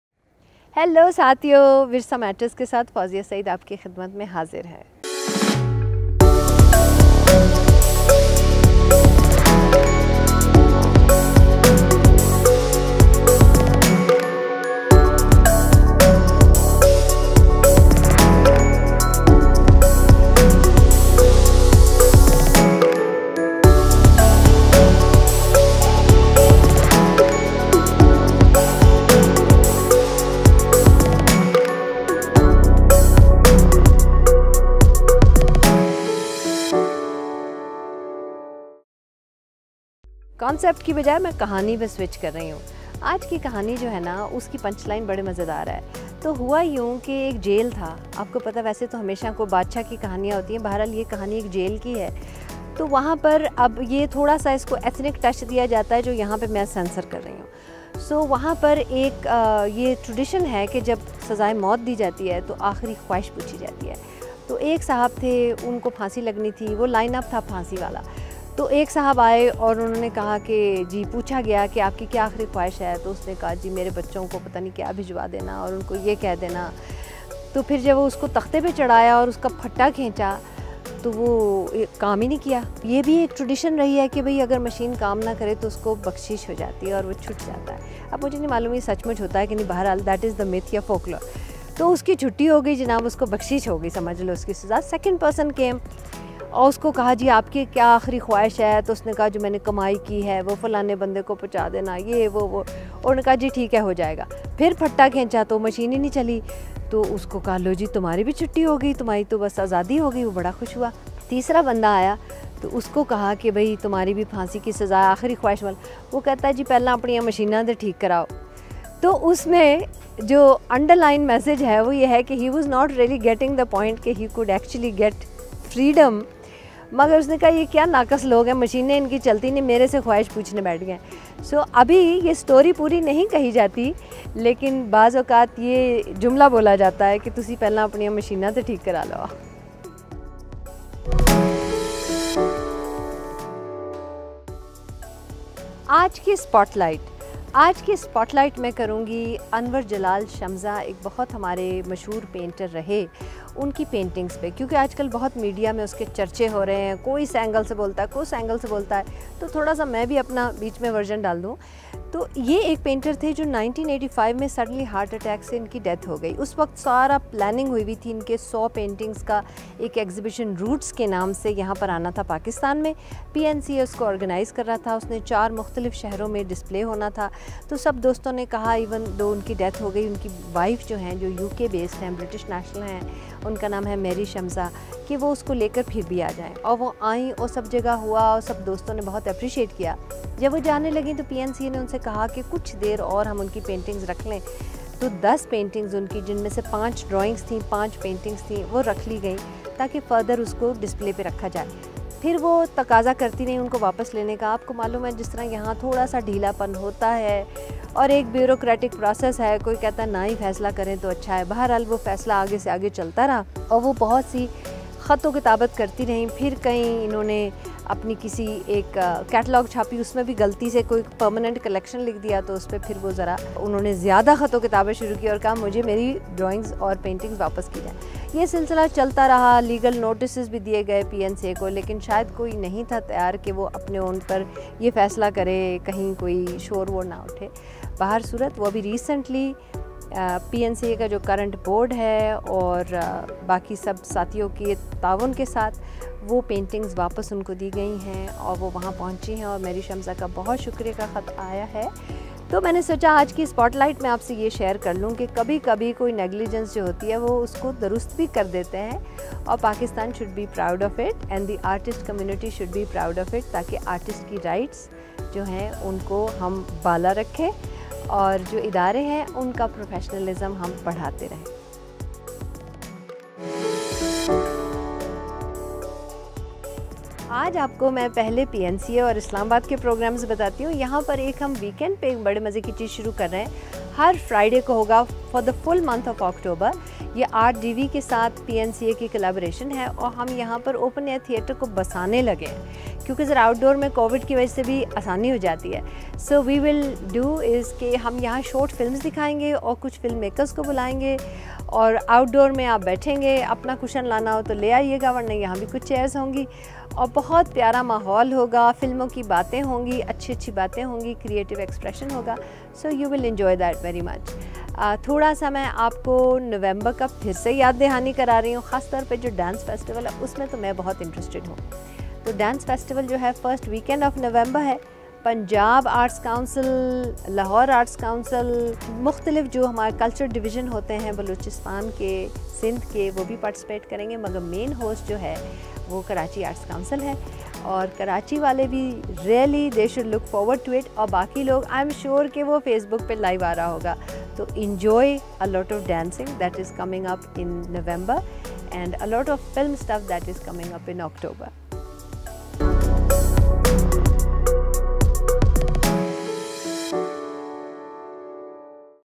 Virsa Matters: How & Why PNCA Returns Shemza's Paintings?
PNCA Board in a recent board meeting decided to return AJ Shemza's paintings to his family after a long struggle of 35 years. Wath Fouzia Saeed, DG, PNCA explain why and how the decision was taken.